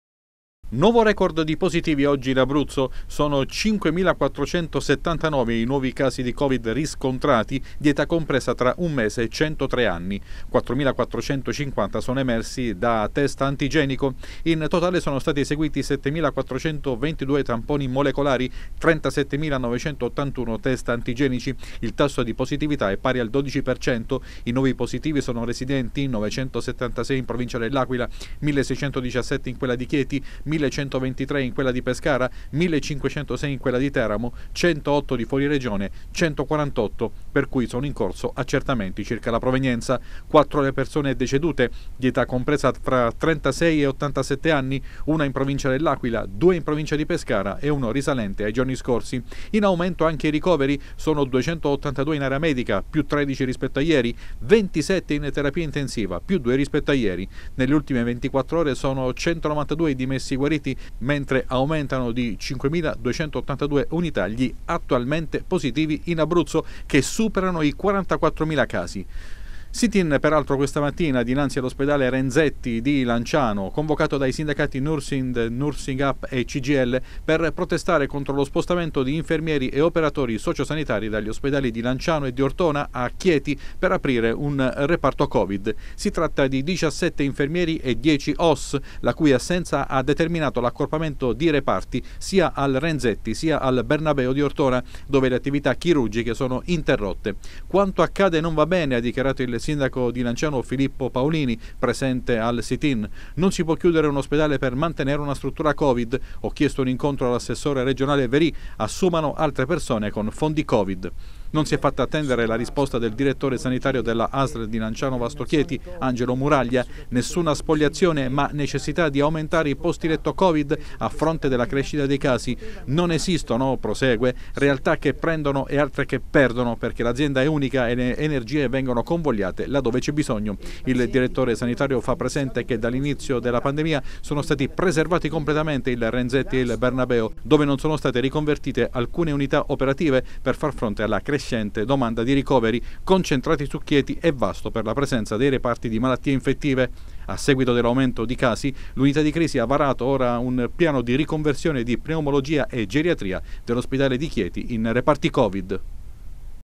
Nuovo record di casi (5479). Polemiche per i nuovi posti letto a Chieti
Sono 5479 i nuovi casi di Covid-19 in Abruzzo, un nuovo record dopo i 5315 di mercoledì scorso. Tra i 4 decessi anche quello di un 36enne. Intanto è polemica per lo spostamento di personale degli ospedali di Lanciano e Ortona nel  nuovo reparto Covid di Chieti